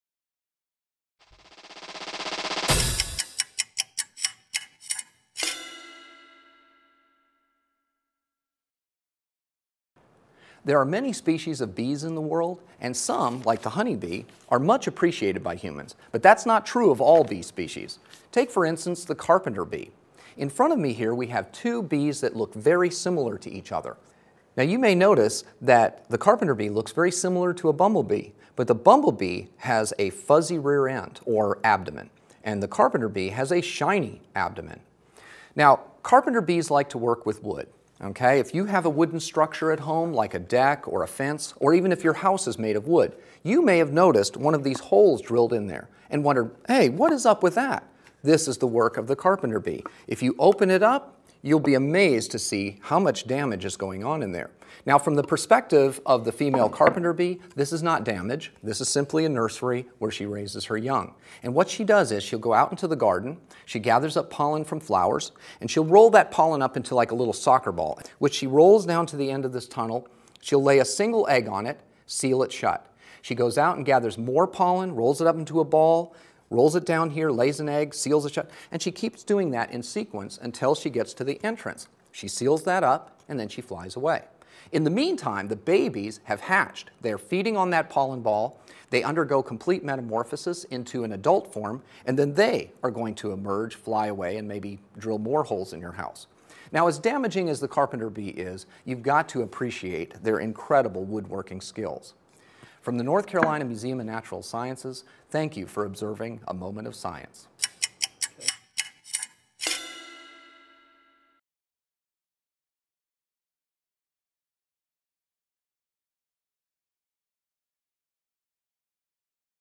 Why Carpenter Bees Drill Holes in Houses: A Moment of Science
Museum educator Bob Alderink gives the inside scoop on the world's first soccer moms, the Xylocopa.